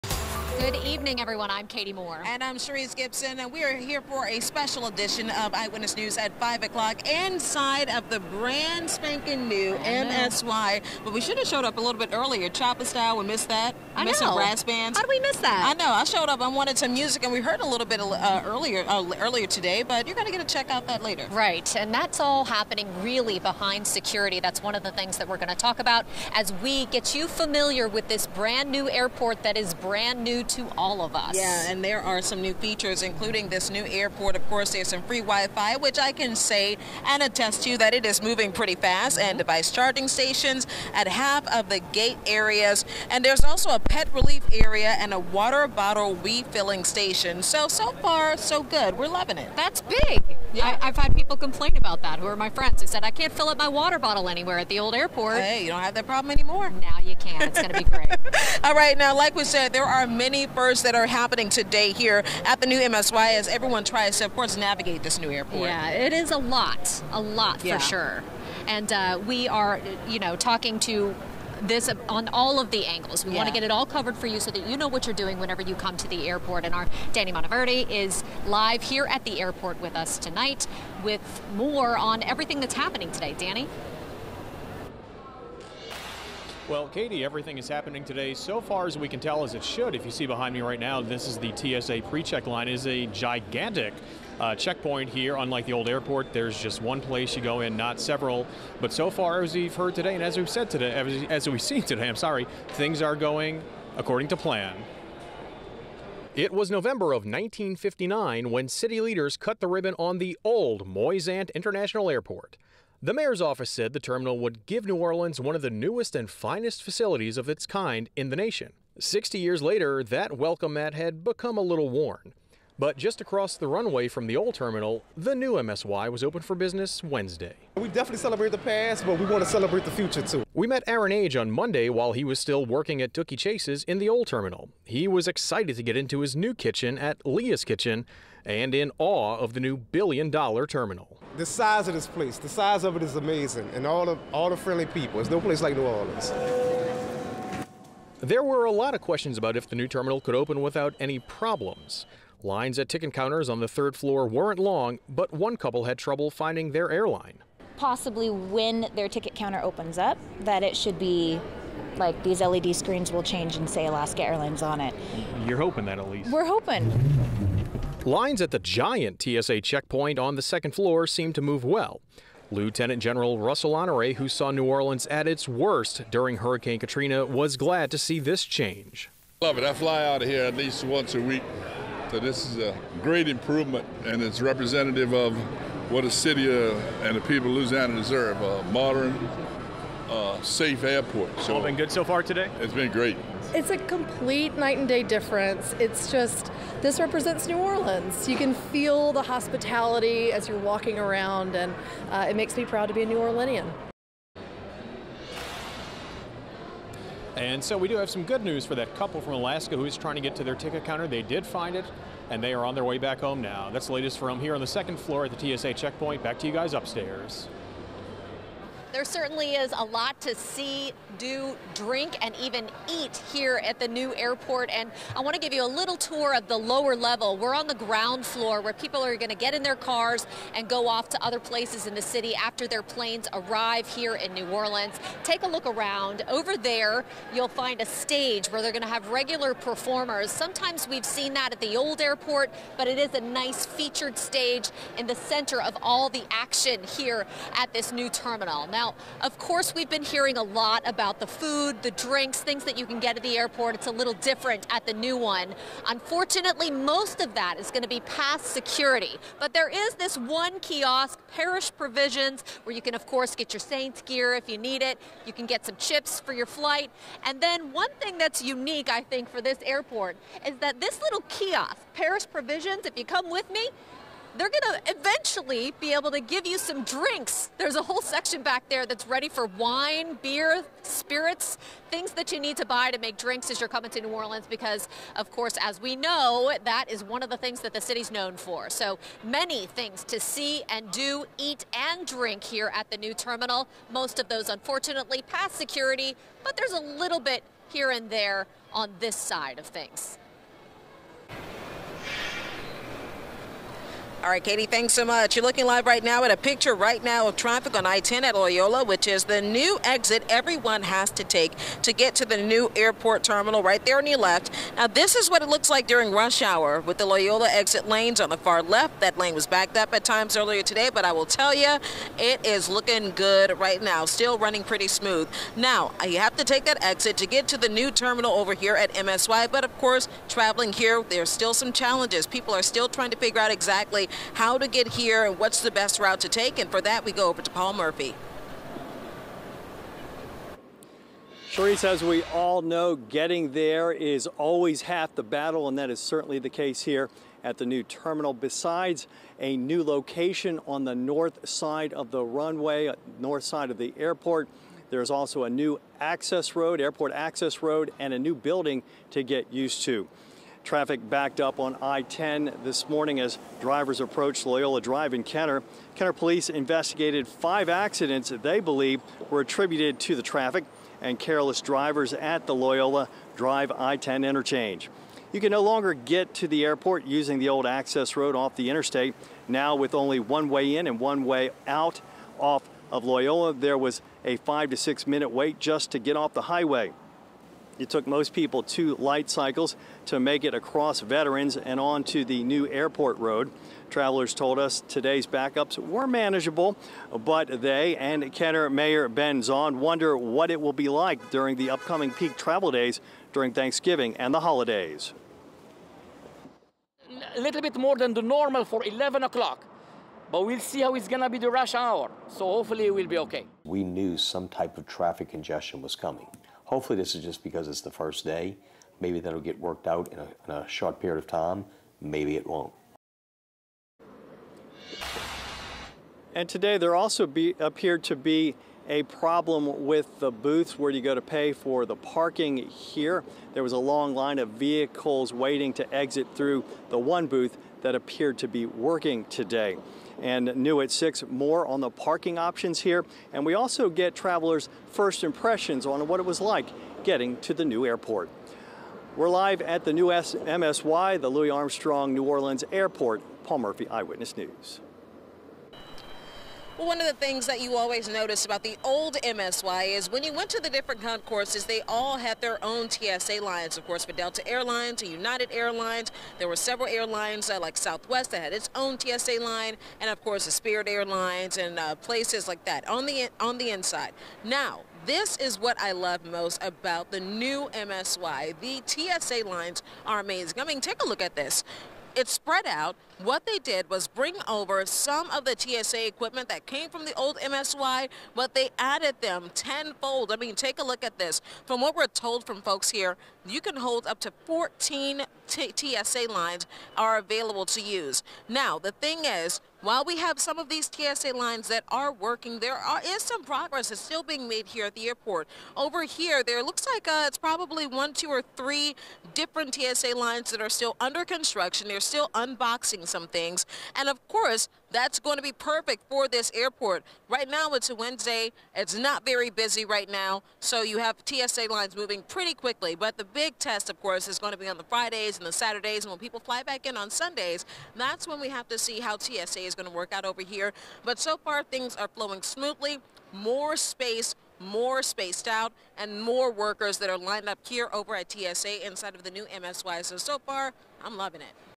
New Armstrong Airport terminal opens - take a look at the new features
The new terminal at Armstrong International airport seemed to be a big hit on the first day, the only problems seemed to be in getting there.

There were 5 crashes near the I-10 exit at Loyola as Kenner Police said those patiently waiting in a five to six-minute wait to get off on the exit had to deal with those who raced up to the exit and then tried to cut over at the last minute.